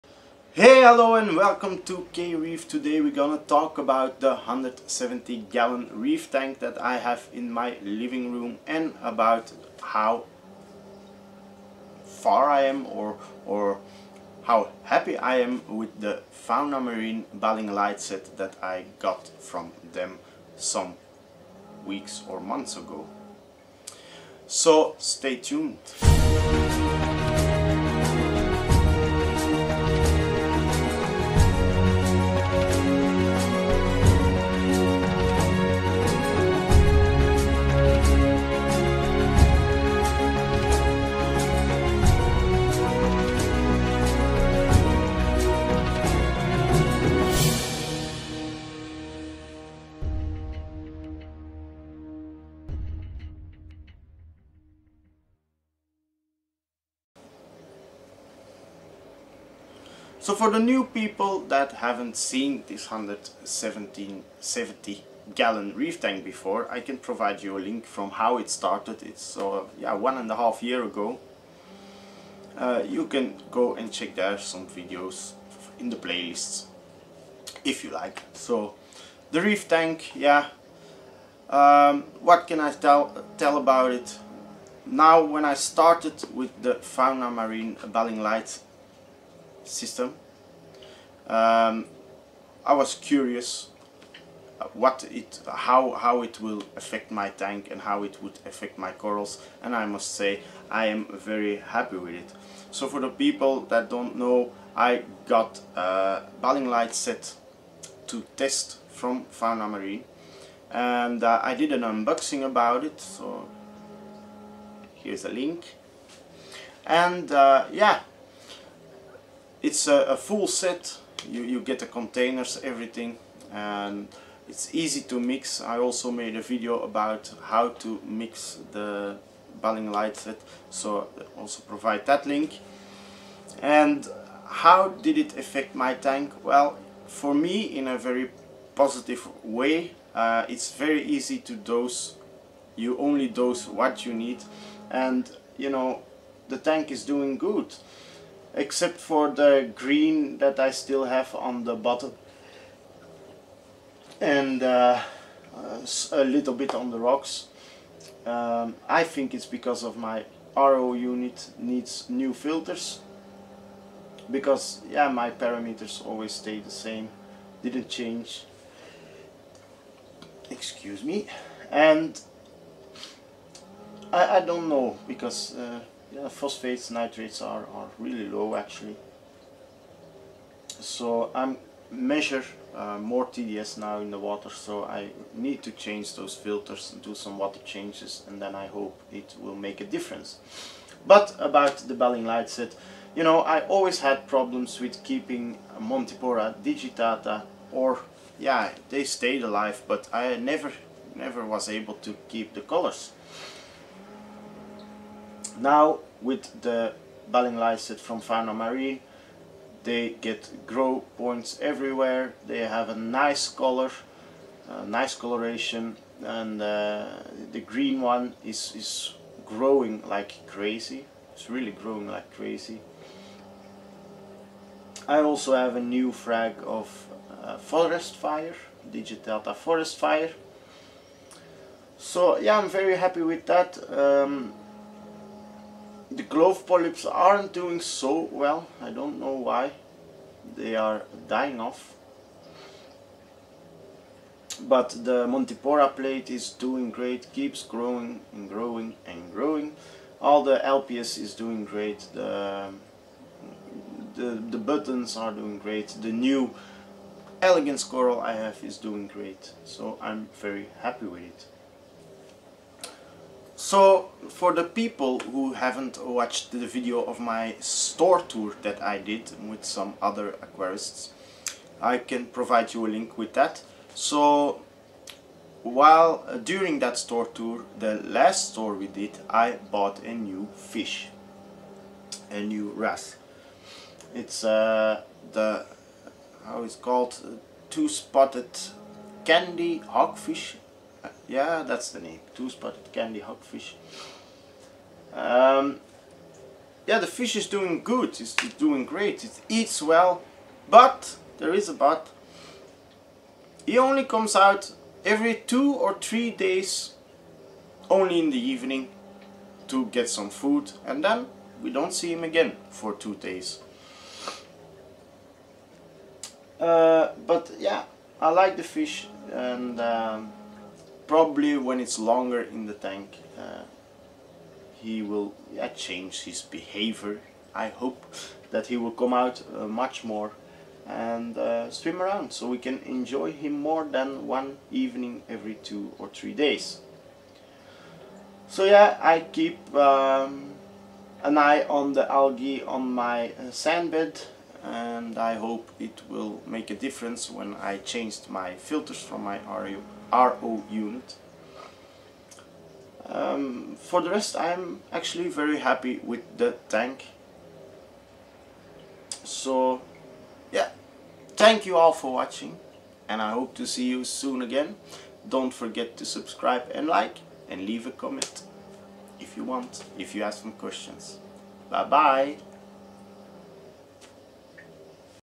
170g Marine aquarium and Balling light Fauna Marin / update 17
How is my reeftank doing with dosing balling light from Fauna Marin?